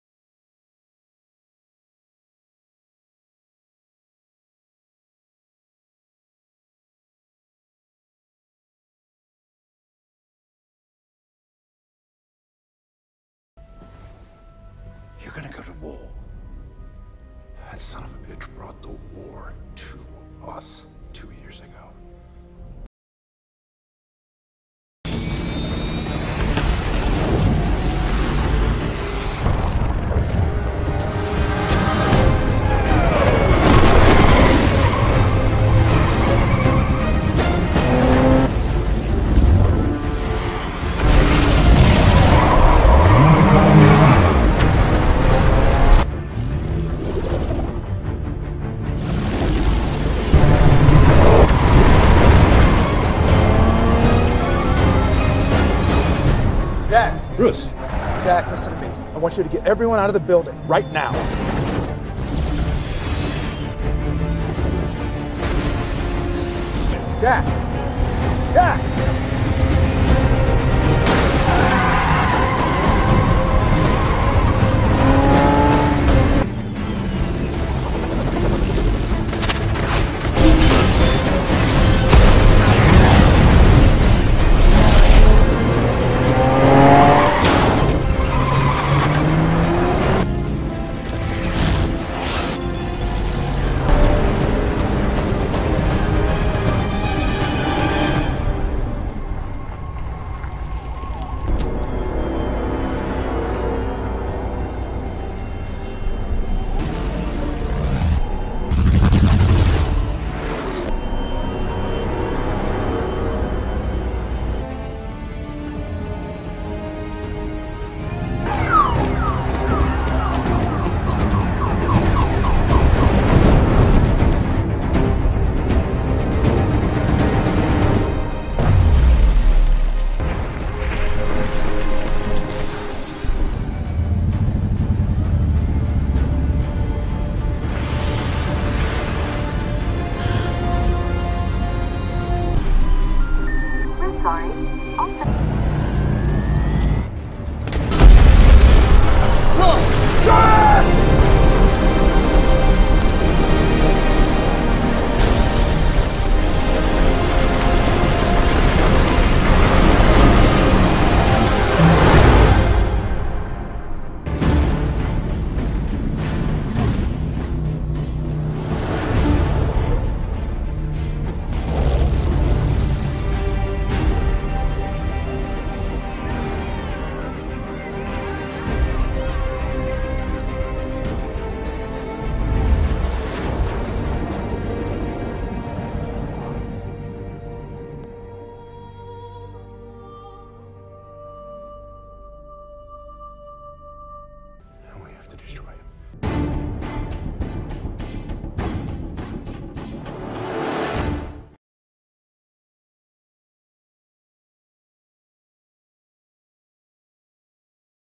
Batwars episode 2: Batman destroys wayne tower- Batman spoof
What would happen if Ben Affleck's Batman and Christian Bale's Batman existed in the same universe? Check out episode 1, then 2 to find out their sad story